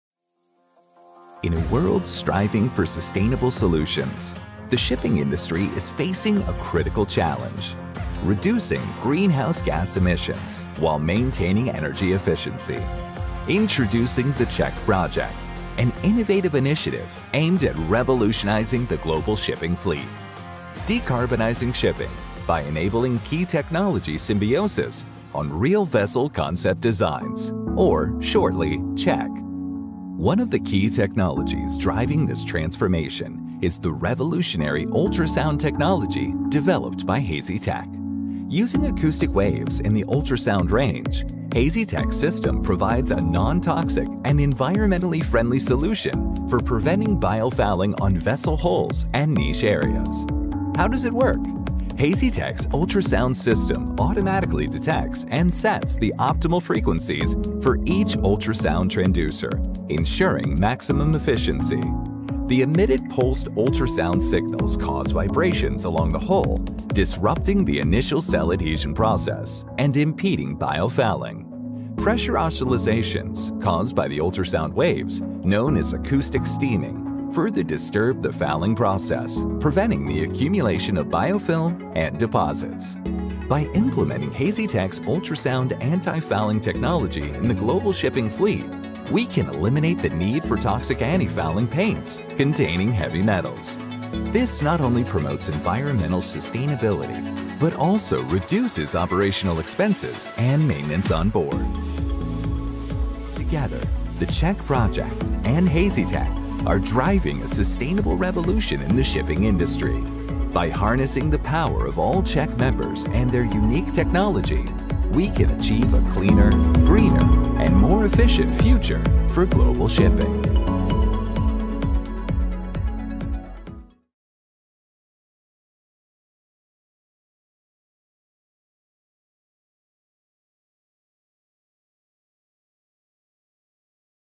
CHEK - Ultrasound Antifouling System - Hasytec
With the implementation of the HASYTEC ultrasound antifouling technology in the global fleet, there will no longer be a need for antifouling paints, typically containing heavy metals. By eliminating the toxic streams, our system promotes environmental sustainability and also reduces operational expenses and maintenance on board.